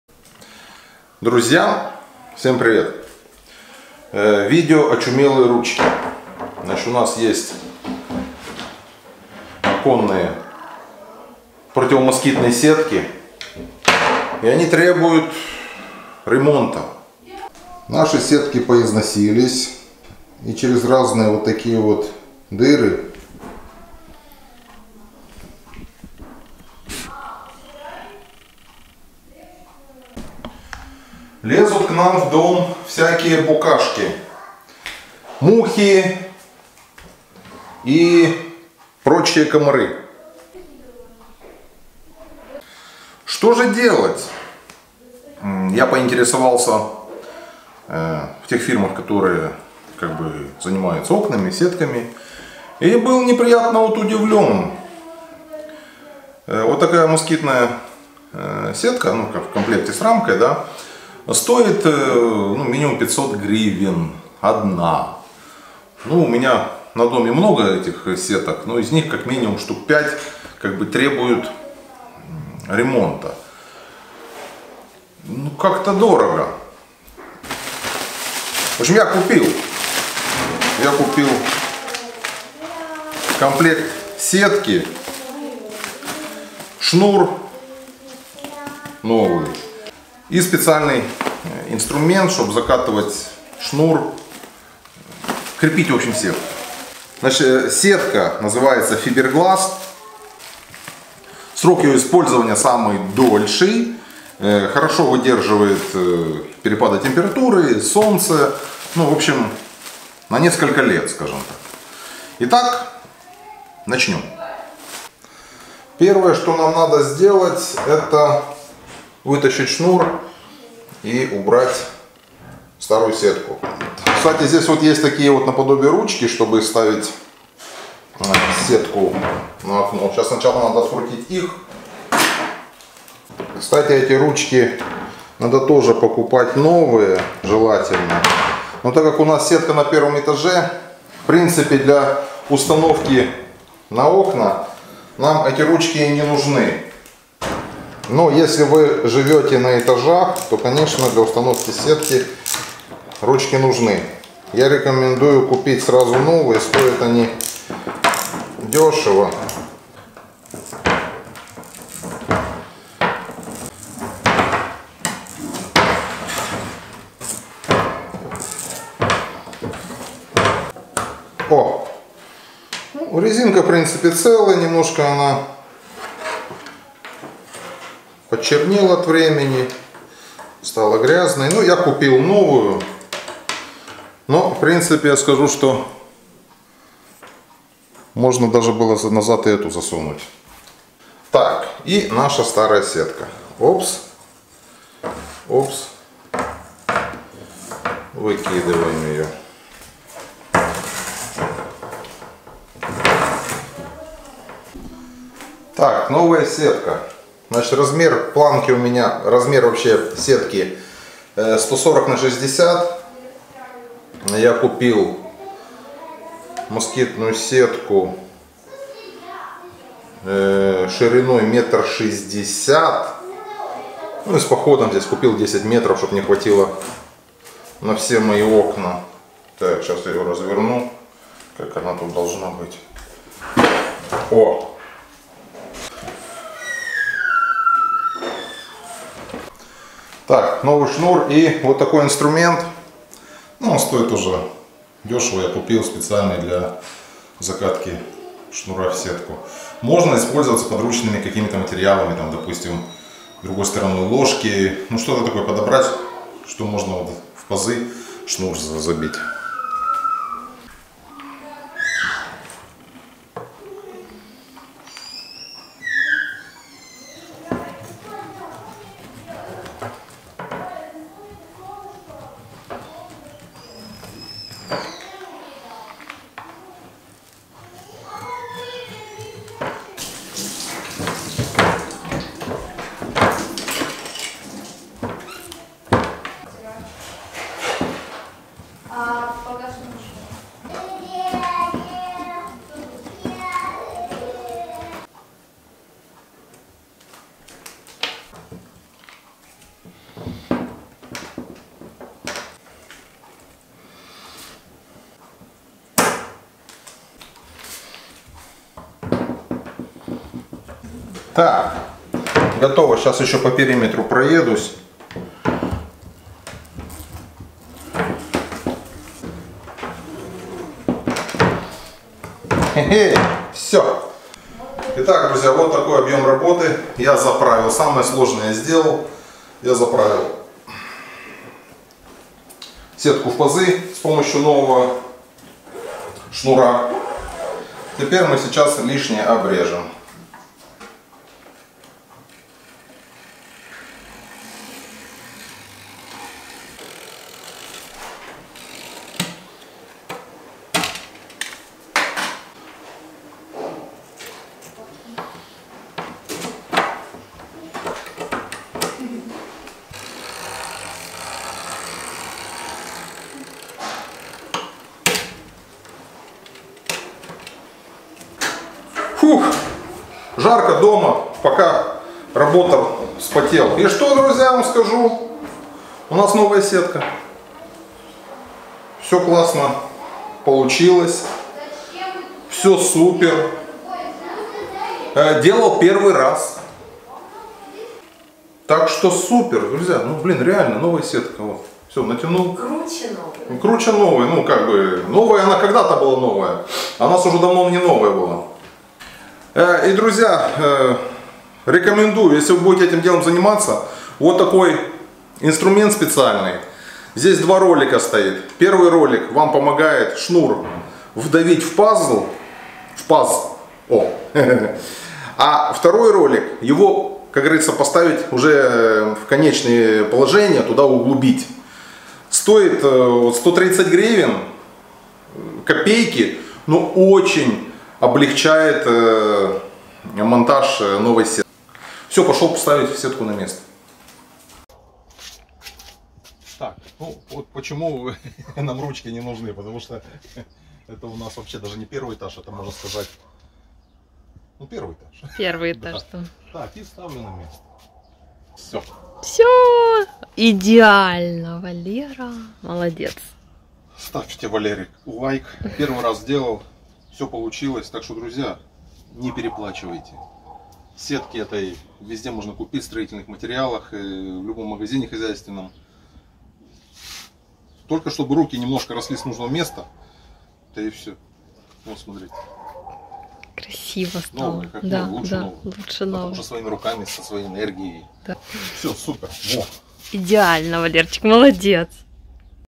Ремонт МОСКИТНОЙ СЕТКИ за 5 Минут!!! Сможет абсолютно любой!!!
Быстрый Ремонт МОСКИТНОЙ СЕТКИ за 5 Минут!!! Сможет абсолютно любой!!! Как отремонтировать москитную сетку.

Жизнь многодетной семьи из Украины! Life of a large family from Ukraine!